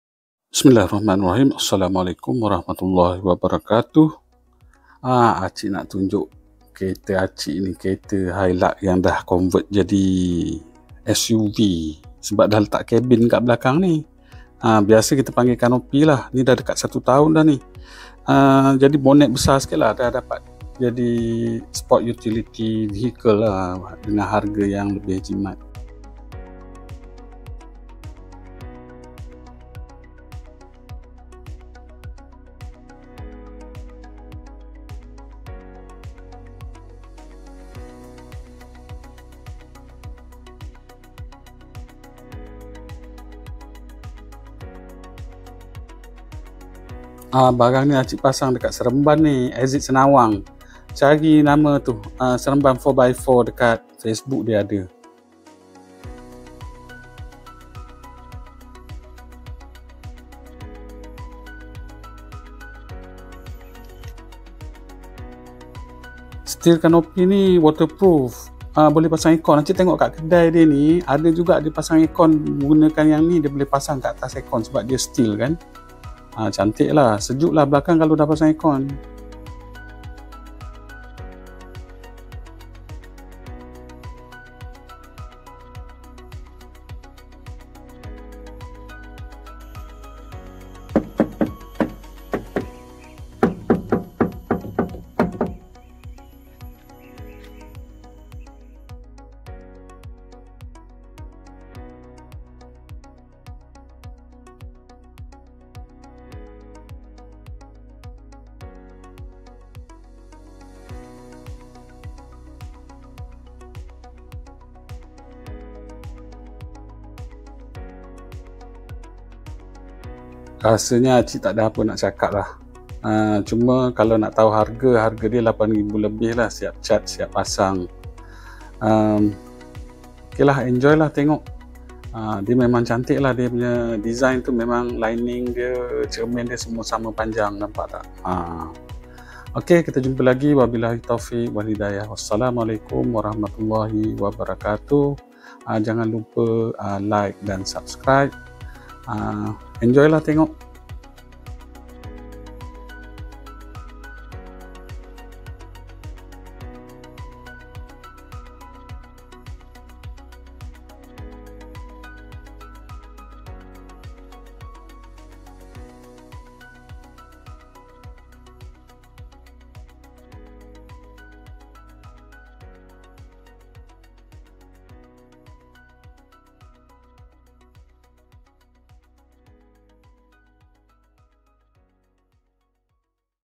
HILUX SUV | INSTALL STEEL CANOPY | SMMITR TL1 | AIR COND | WATER PROOF | MALAYSIA
Hilux Rouge install Thailand made SMMITR Steel Canopy, model TL 1.  Become Hilux SPort Utility Vechicle, SUV.  Worth it, can install air condition too.
hilux gr sport